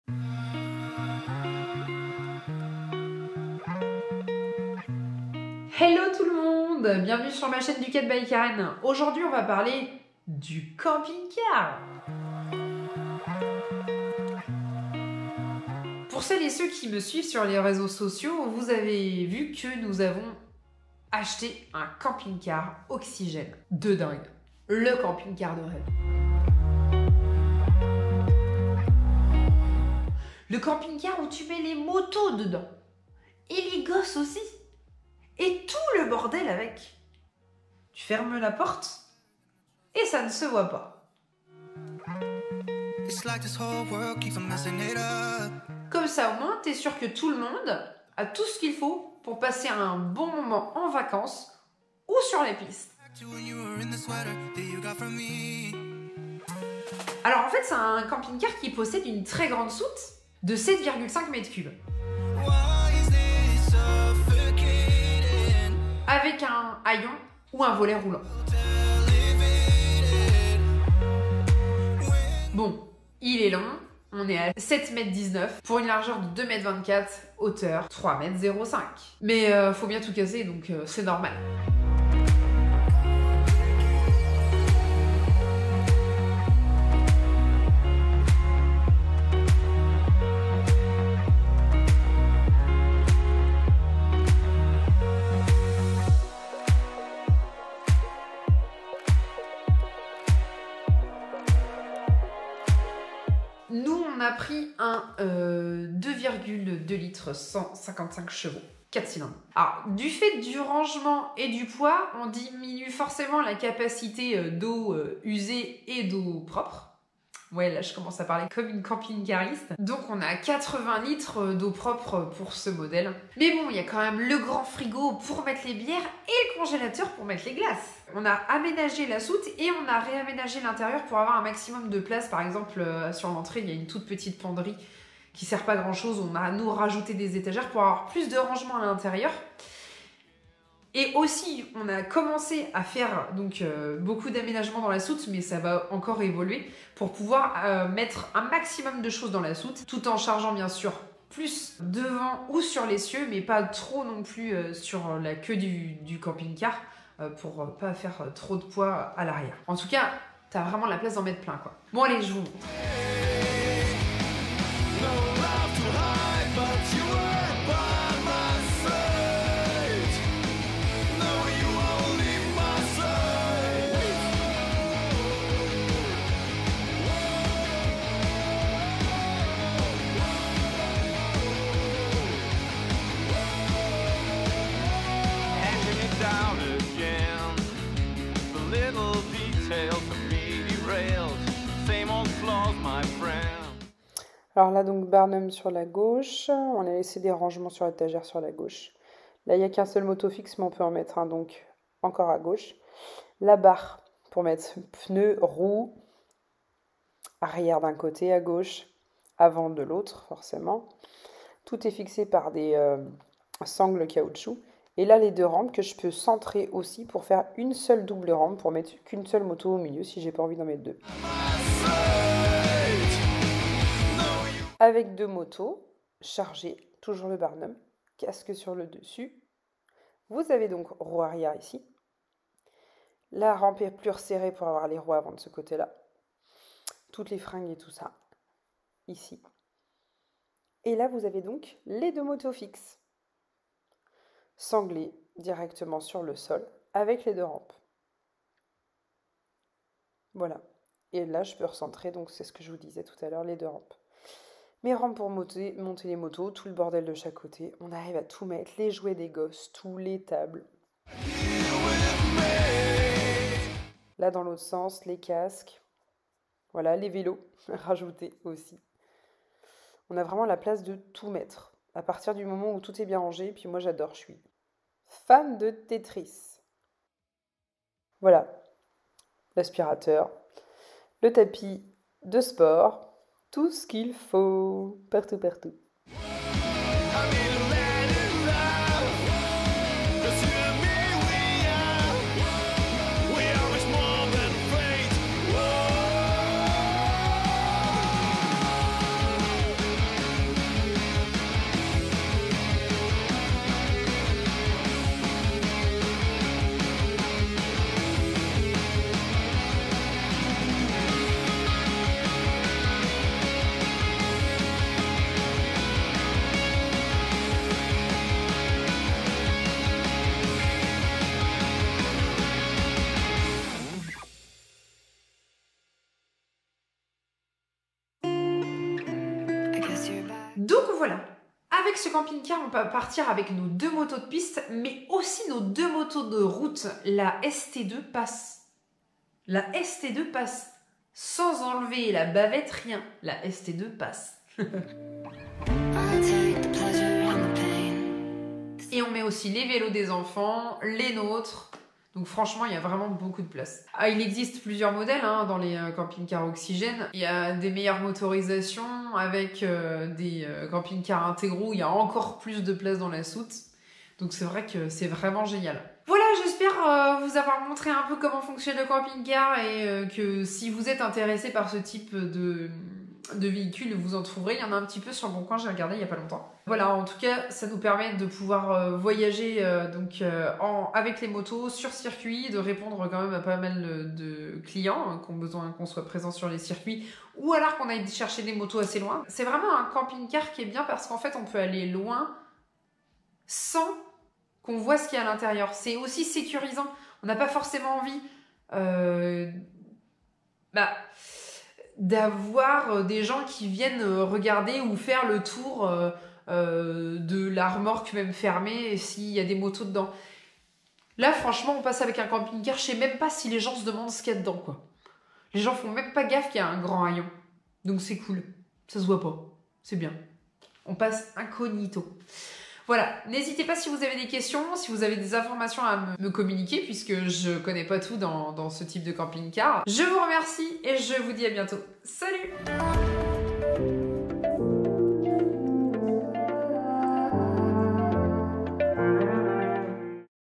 Le camping car pour les motos!
Voilà une vidéo que vous attendez avec impatience.

Voici LE camping car qui peut accueillir tes motos sans que cela ne se voit. Conçu pour les sports mécaniques, c'est le camping car de rêve!!